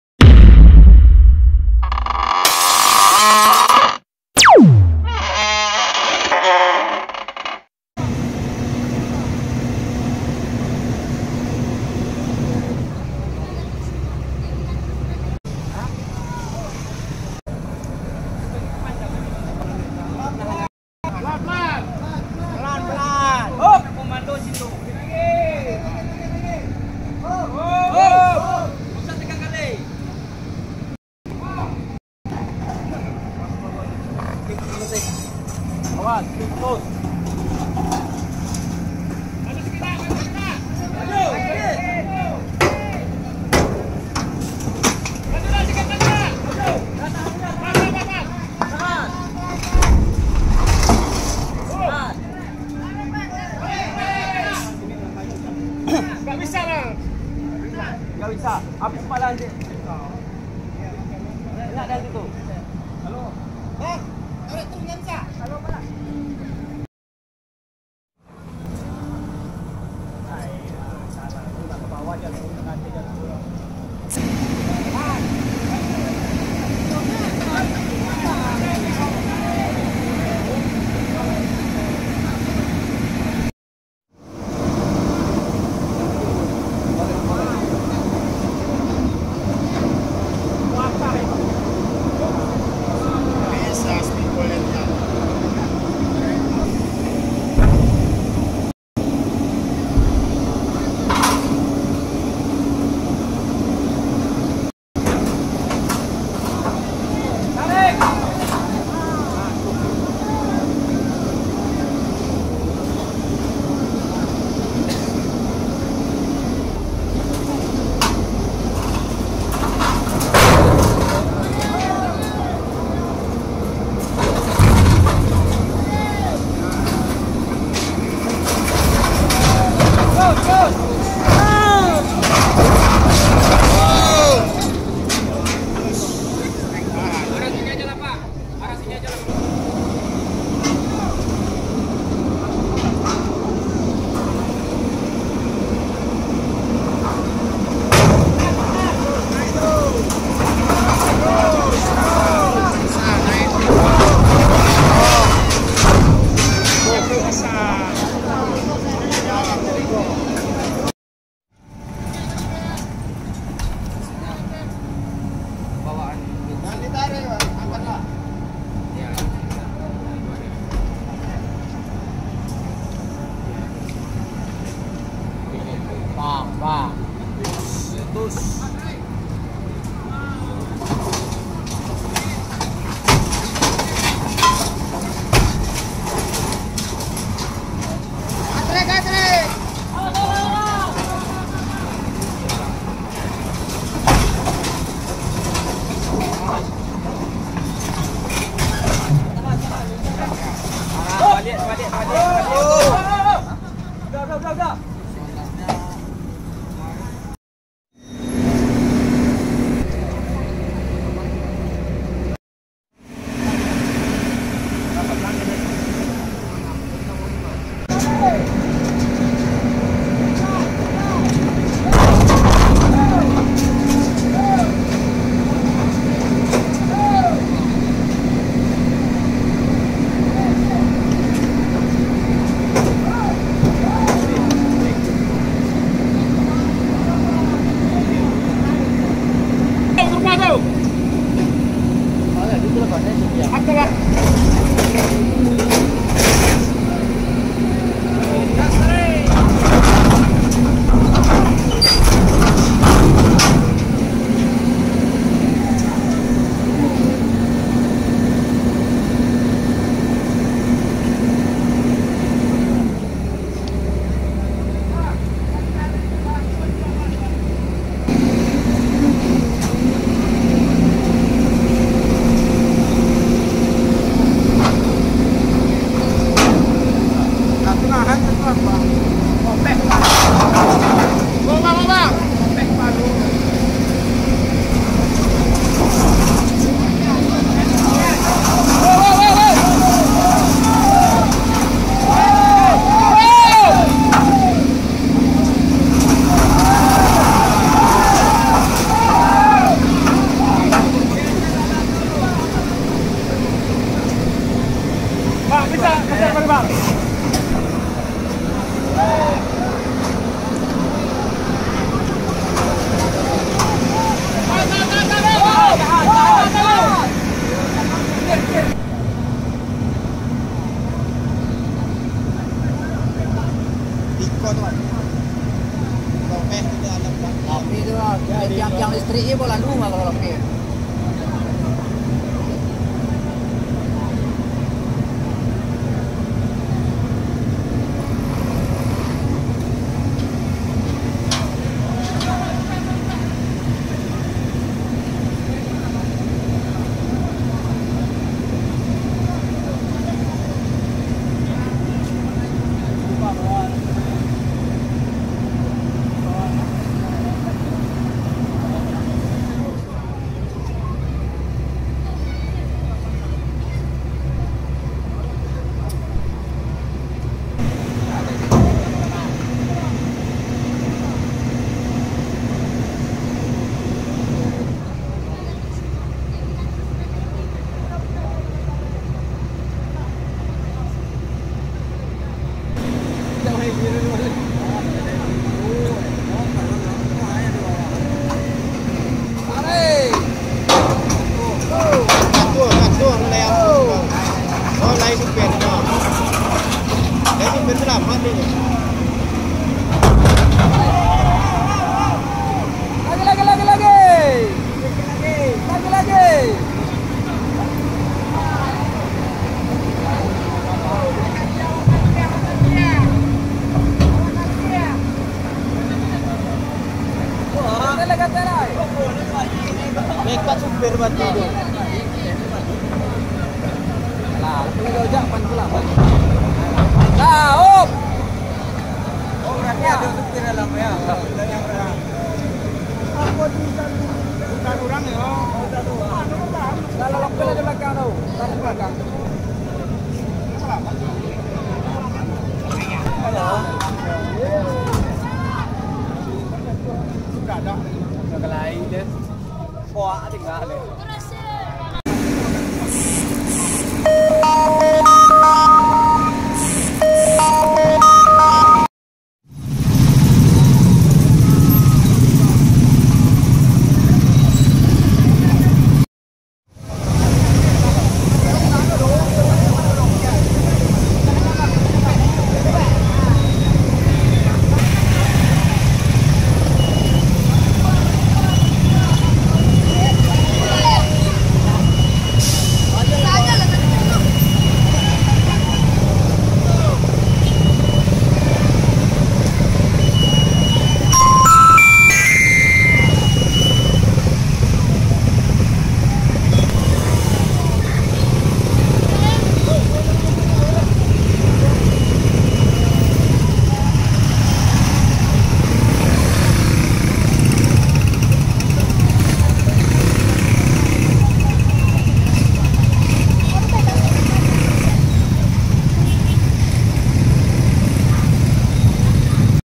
KECELAKAAN MAUT, MOBIL TRONTON TABRAK JEMBATAN SUNGAI ROKAN, UJUNGBATU
Mobil Tronton angkut sawit terjungkal ke jurang dekat sungai rokan Ujungbatu, Rokan hulu, Riau